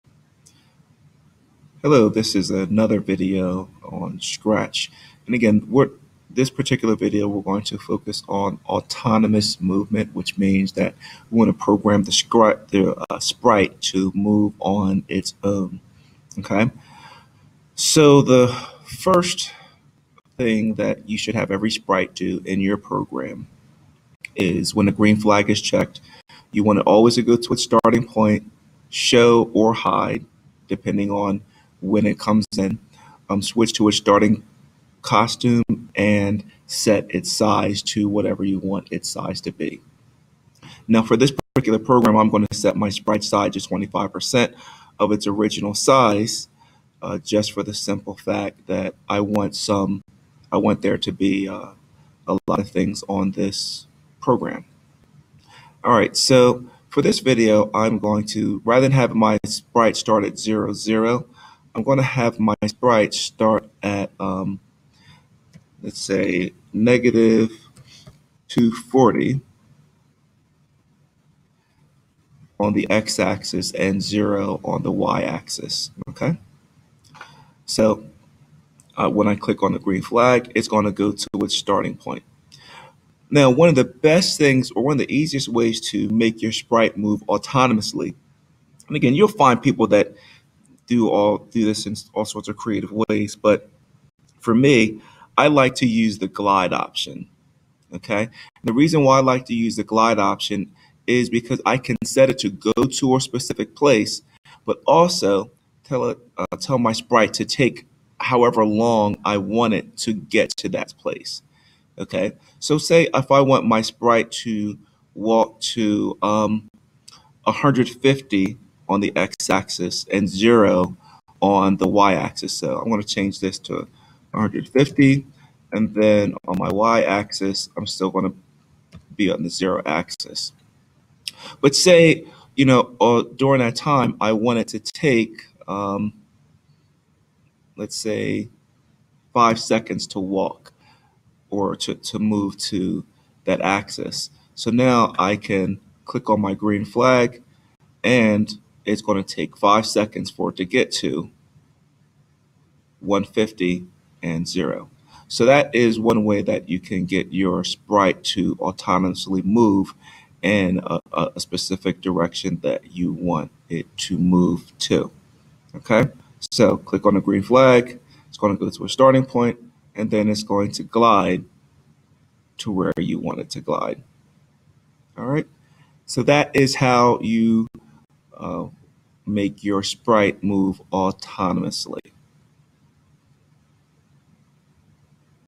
Making your sprite move by itself in Scratch
This video demonstrates how to make a sprite move autonomously (By itself).  Gliding in one direction